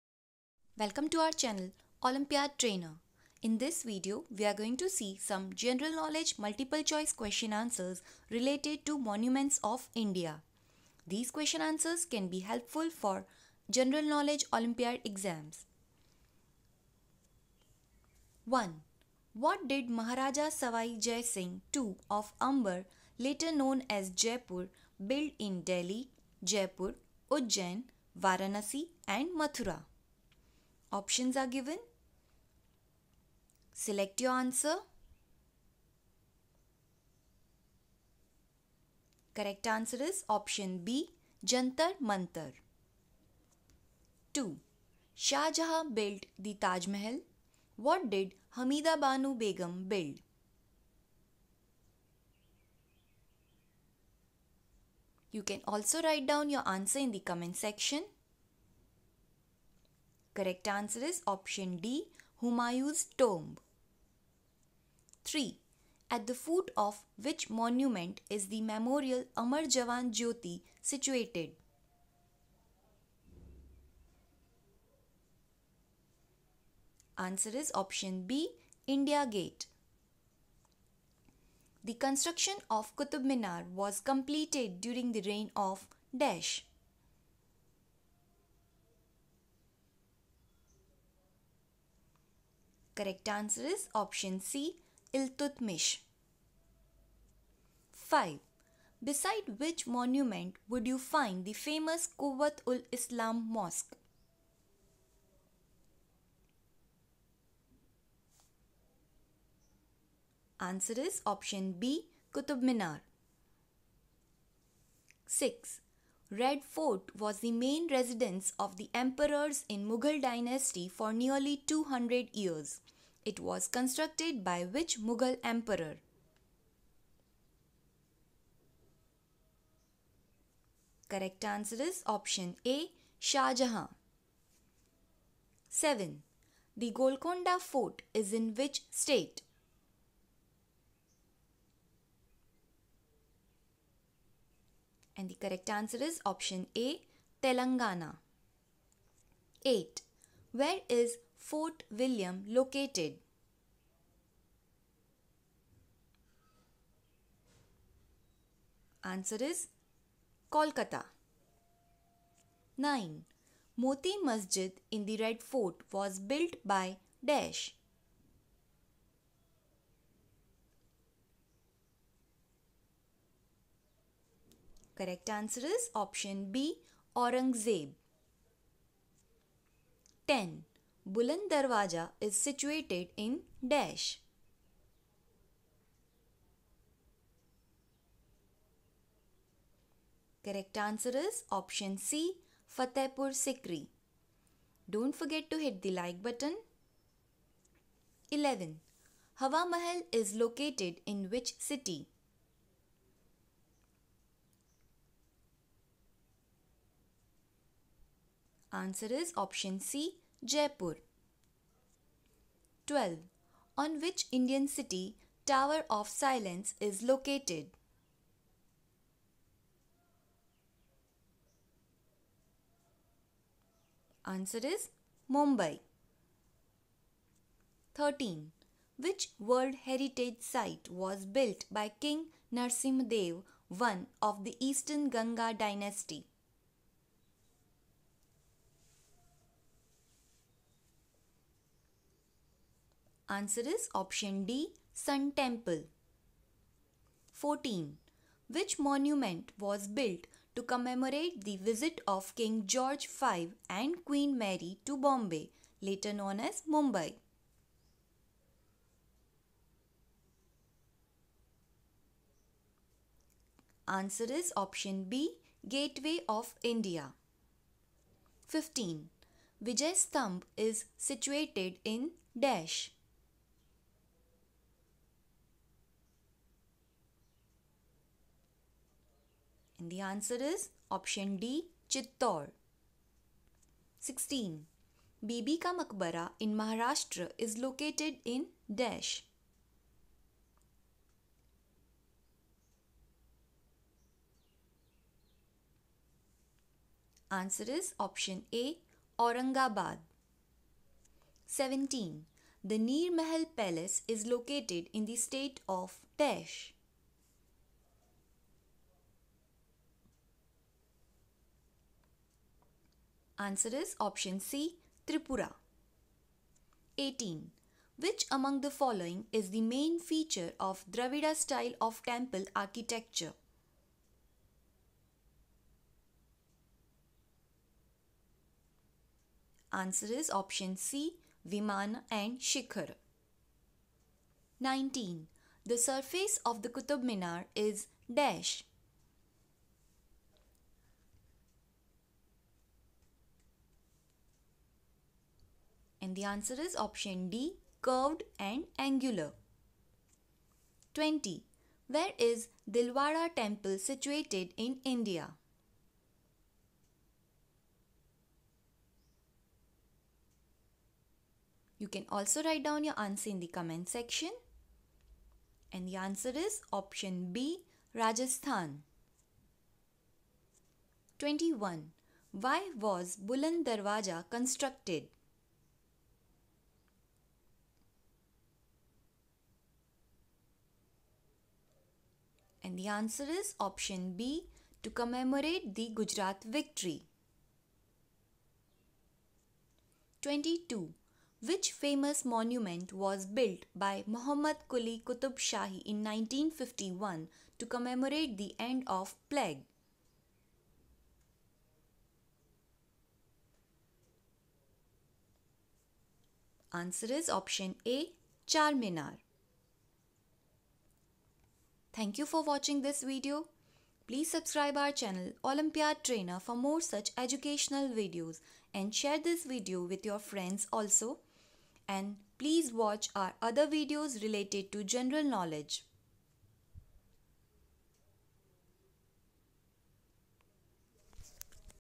IGKO Class 7/ 8/ 9 "Monuments of India" General Knowledge Olympiad
General Knowledge Olympiad question answers related to Monuments of India.